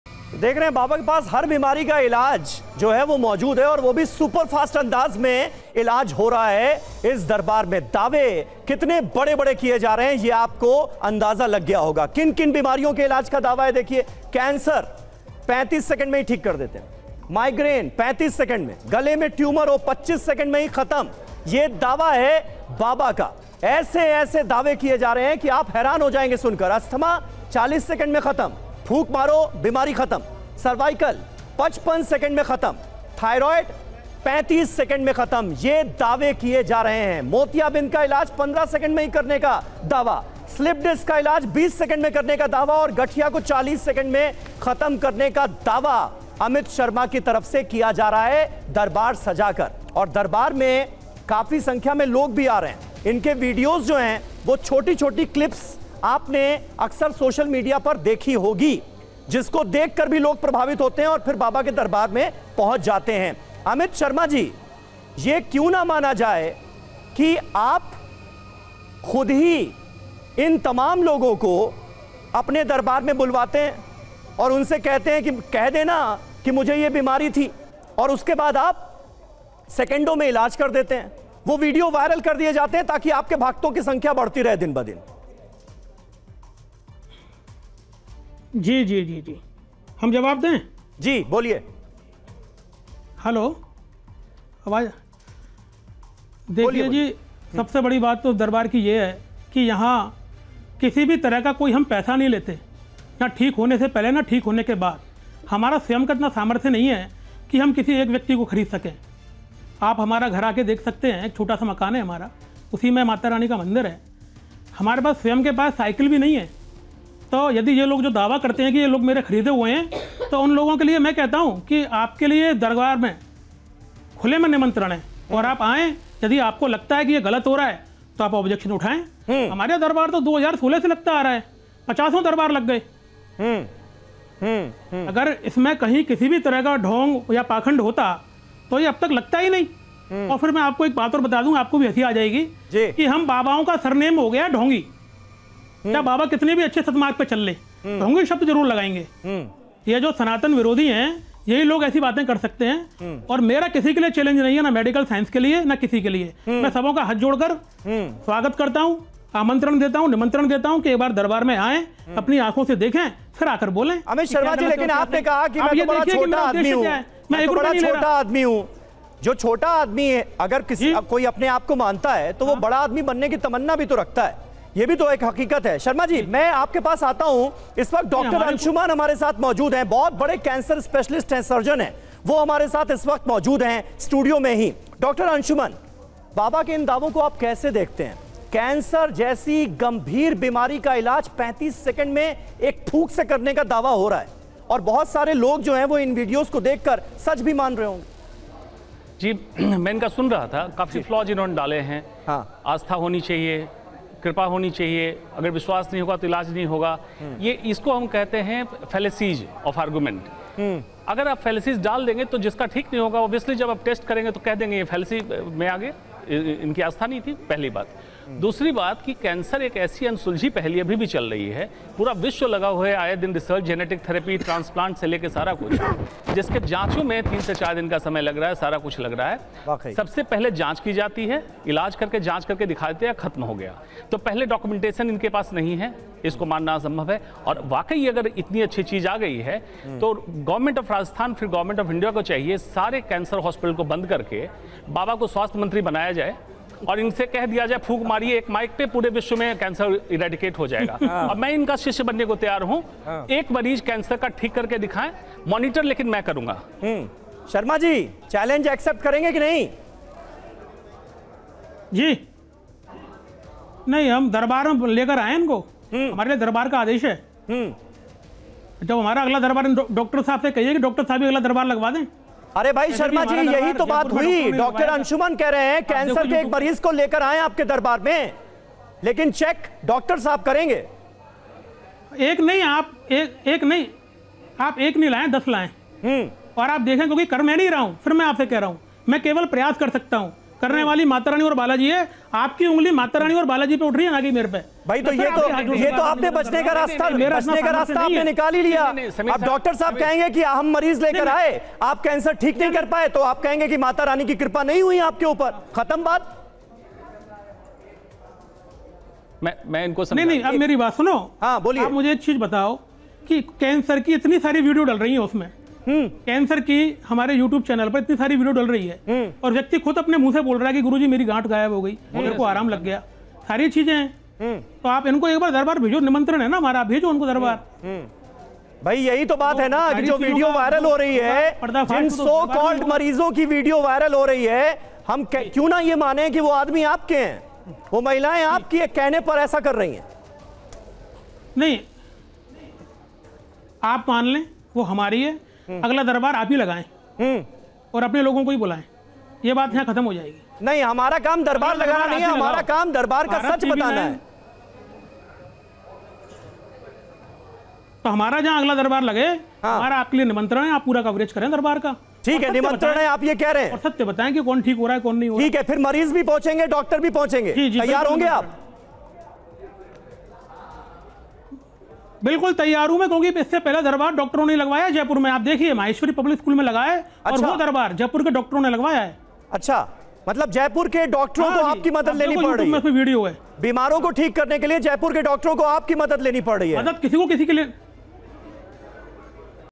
Baba Baglamukhi का किन-किन बीमारियों के इलाज का दावा? | Amit Sharma | Rajasthan | Viral Video
डॉक्टर बनने के लिए पहले प्रतियोगी परीक्षा की तैयारी करनी पड़ती है. फिर 5 साल का एमबीबीएस और फिर स्पेसलाइजेशन का कोर्स करना होता है. फिर जाकर कोई डॉक्टर मरीज का इलाज करने योग्य बनता है. लेकिन राजस्थान के भरतपुर में एक बाबा जी ऐसे हैं जो बड़े अजब गजब हैं. जानते हैं क्यों...क्योंकि ये कैंसर से लेकर एड्स, पैरालिसिस से लेकर डायबिटीज, माइग्रेन से लेकर स्लीप डिस्क, हर तरह की बीमारी को ठीक करने का दावा करते हैं'.